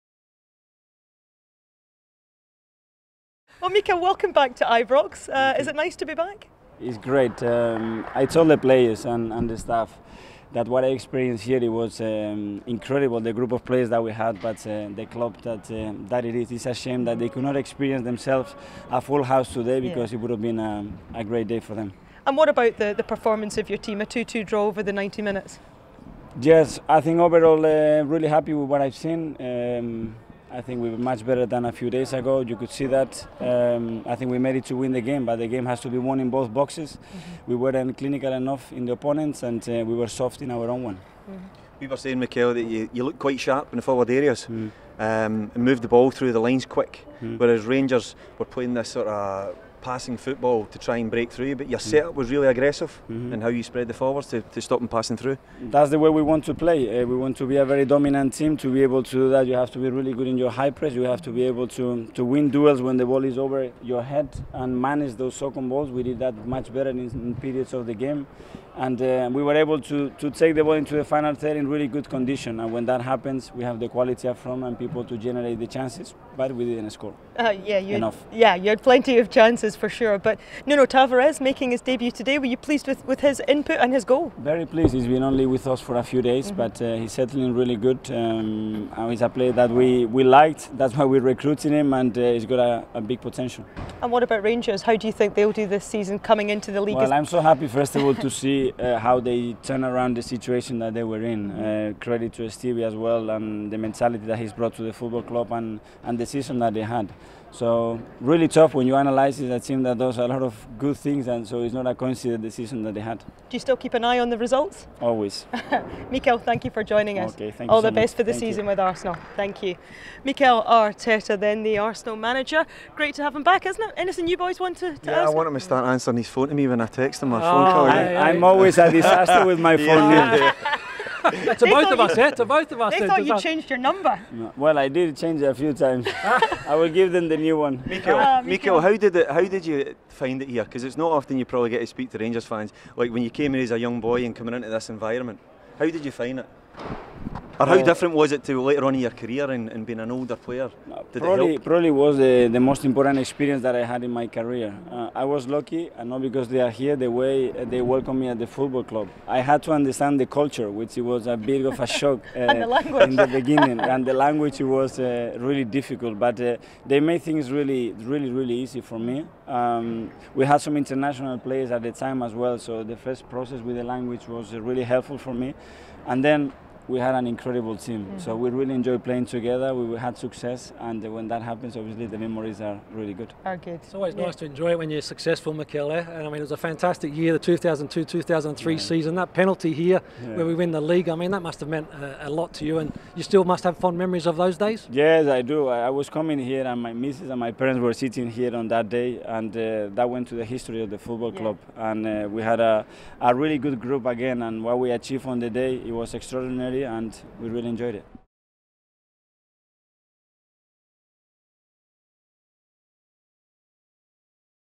REACTION | Mikel Arteta | Rangers v Arsenal 17 Jul 2021
RangersTV spoke to former Ranger Mikel Arteta after his Arsenal side drew 2-2 at Ibrox.